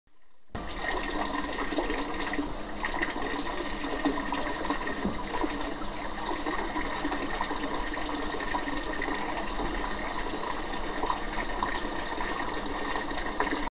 Brat bath
This is my pet indoor white chinese goose Brat. She likes the bathtub sometimes becuse I can get i deep enough to float in. Normaly she plays in her pool outside.